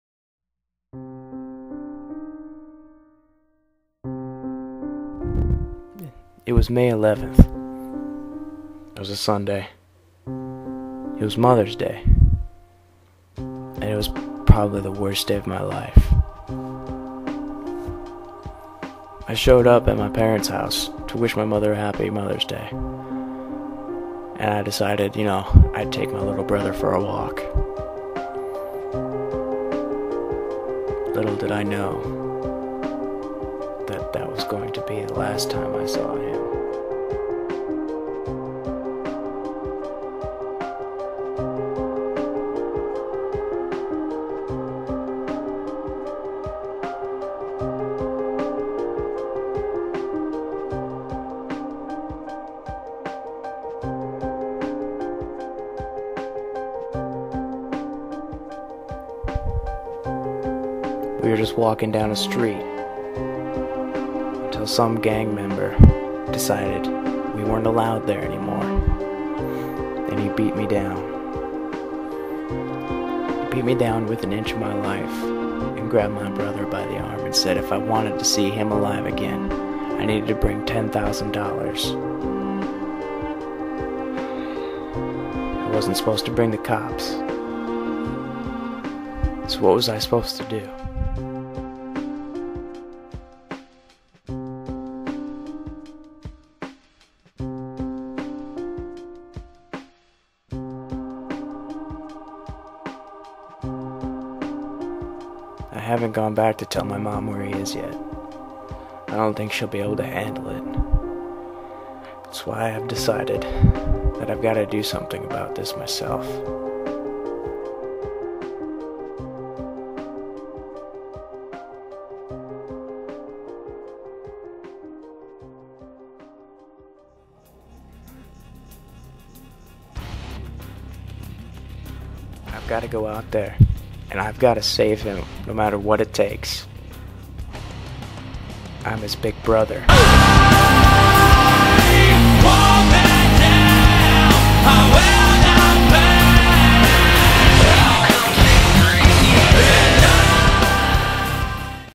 Big Brother Trailer
I recently watched the movie Death Sentence starring Kevin Bacon and came up with this little idea. The movie kicked ass and really was heart felt in my opinion. This WILL be made. I promise.